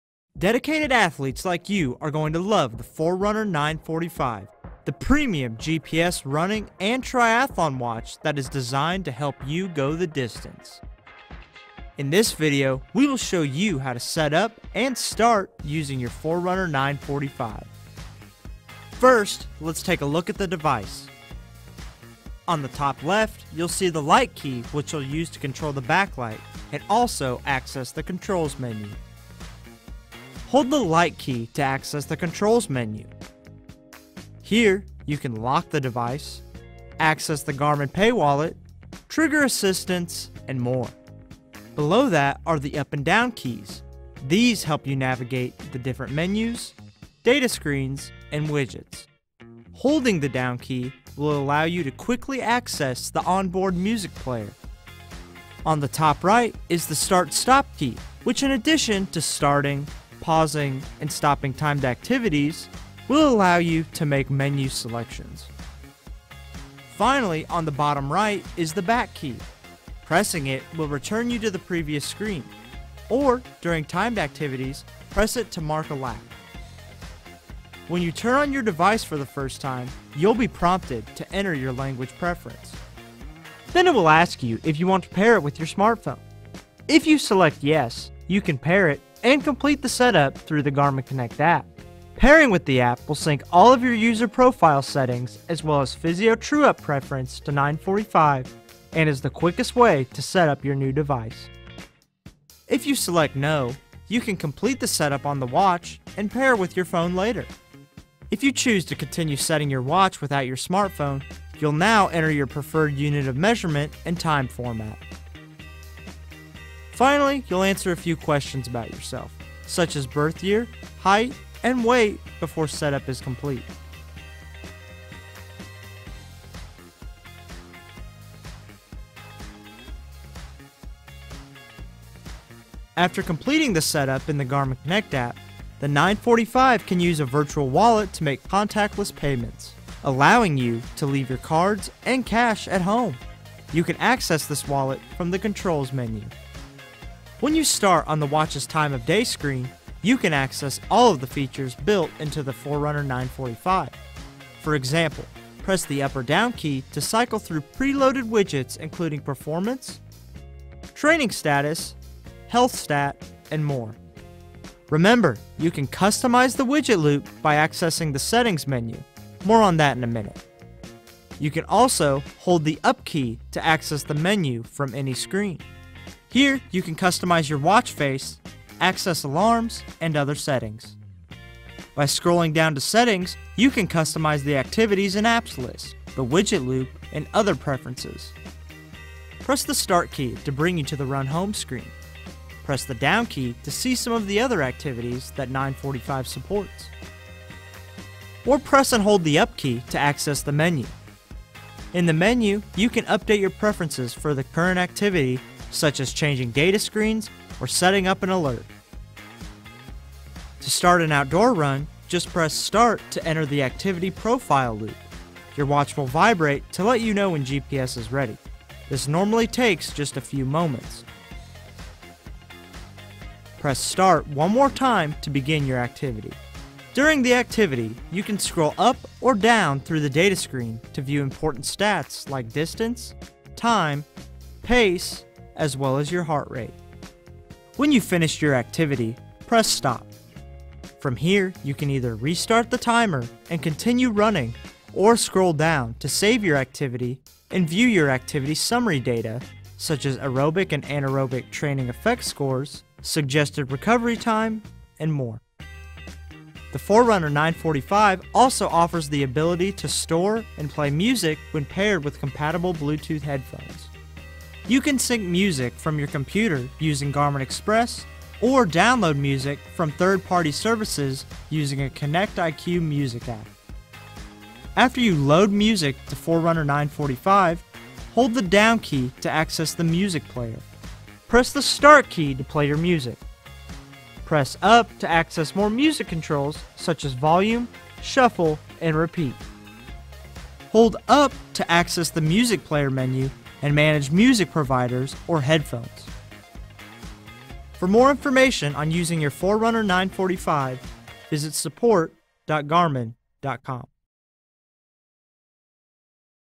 【教學】Forerunner 945: 開始使用
這支影片將會將您如何使用Forerunner 945